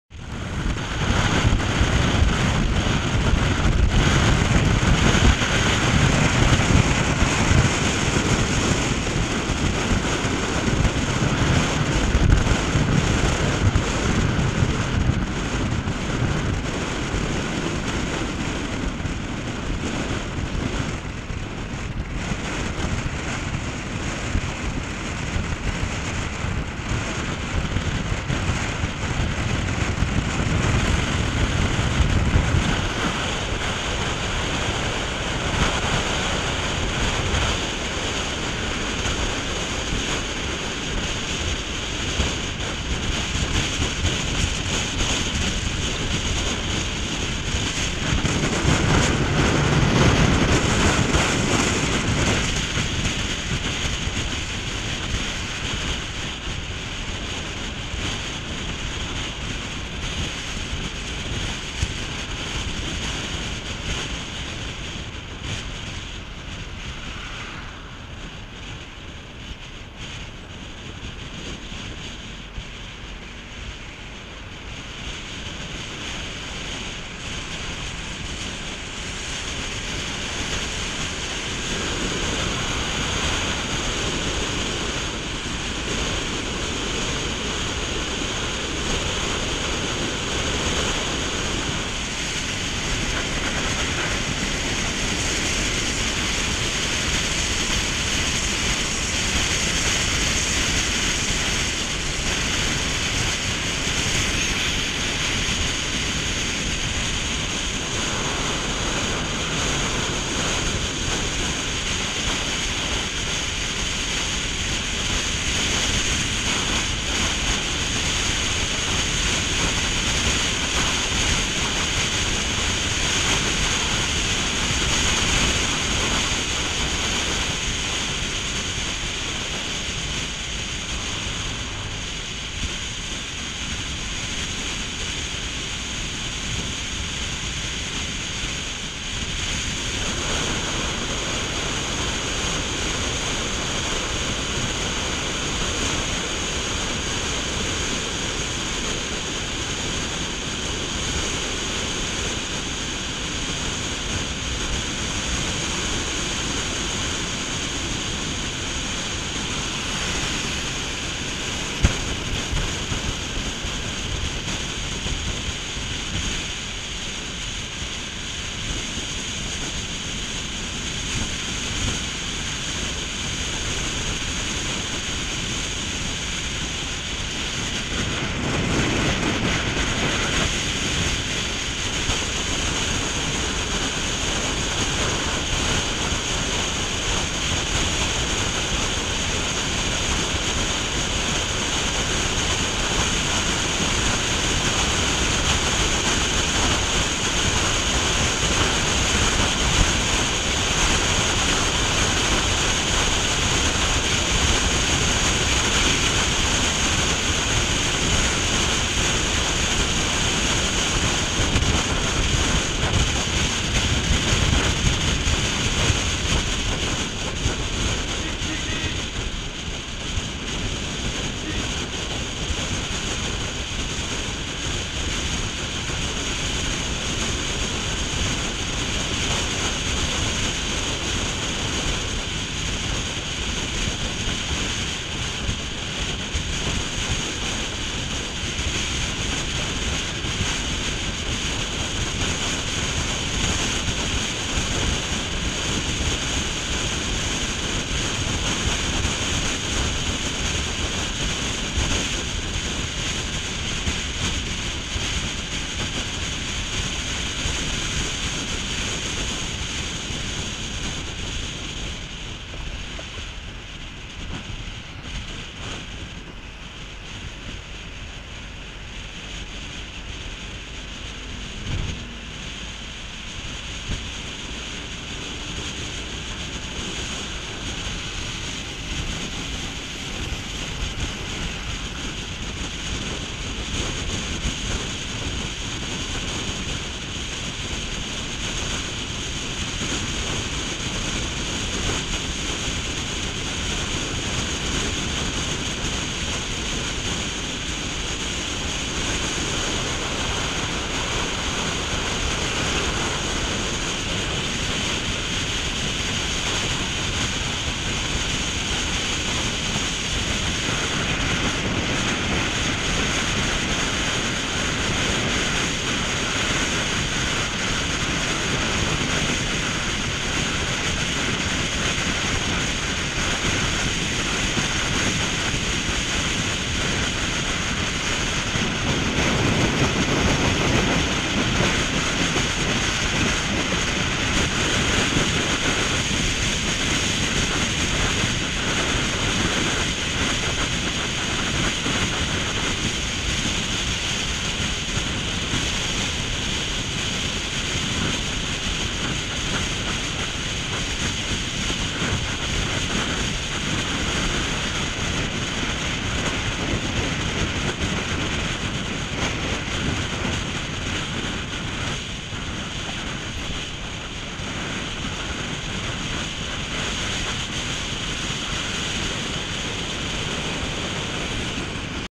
SRP Cebu, Philippines 2014
A run along the new development on the SRP in Cebu, Philippines. Very slow development as I have been away nearly a year and can see only about 1 months worth of construction change.